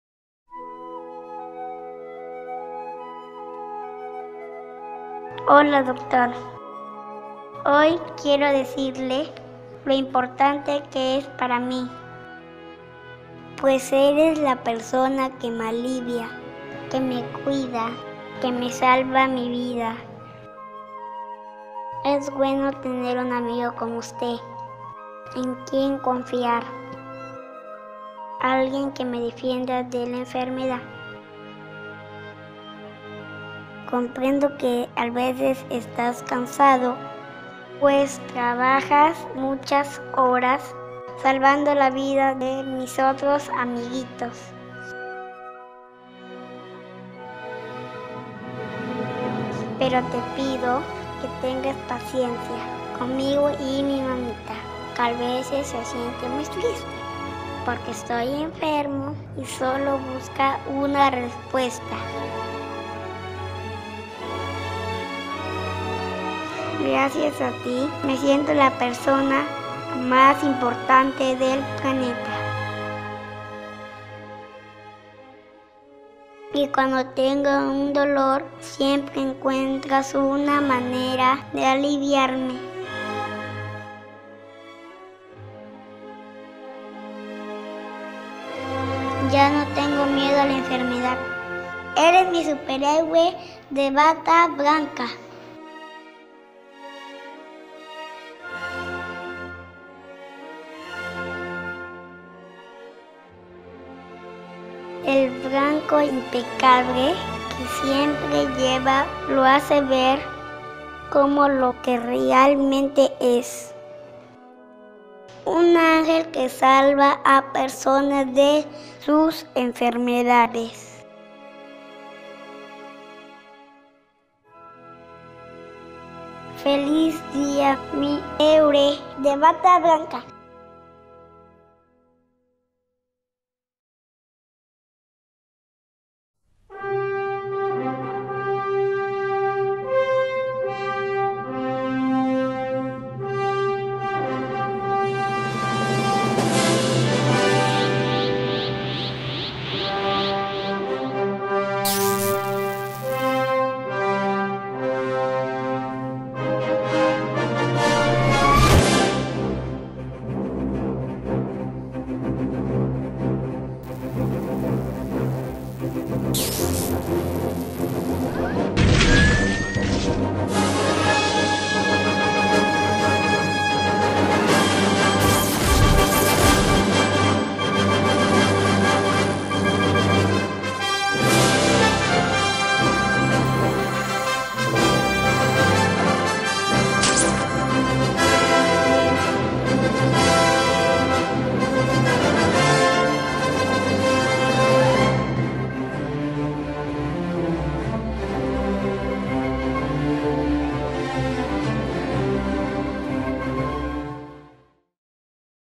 Día de la Medicina Peruana - Instituto Nacional de Salud del Niño San Borja
Video en homenaje a todos los médicos en el día de la medicina peruana-